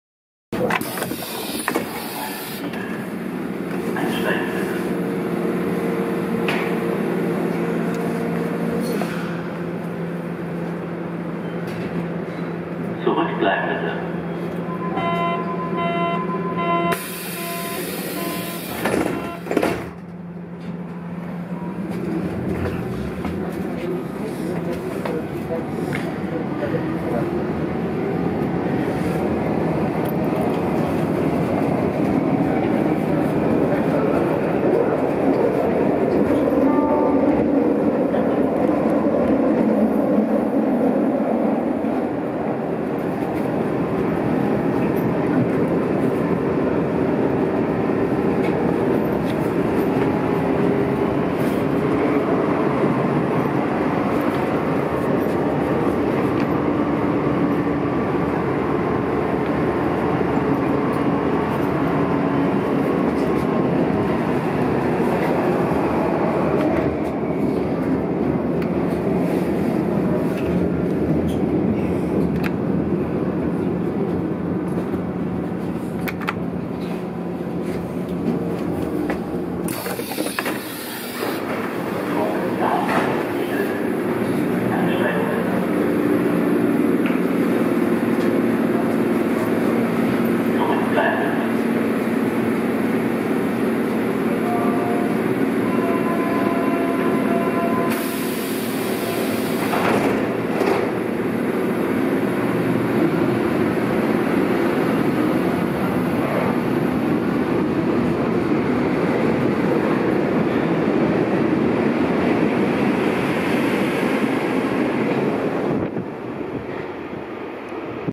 U-Bahn Berlin Mitfahrt im F92 auf der U6 mit Ingo und Helga im Jahr 2022 [1080p50 ᴴᴰ]
Hier ist eine Aufnahme von gestern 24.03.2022. Da war ich positiv überrascht dass Helga immernoch Drehstromer gibt. Eigentlich haben das Drehstromer nicht mehr aber hatte ich gestern ein Drehstromer mit Ingo und Helga. Es gibt tatsächlich noch ein paar Drehstromer mit Helga und Ingo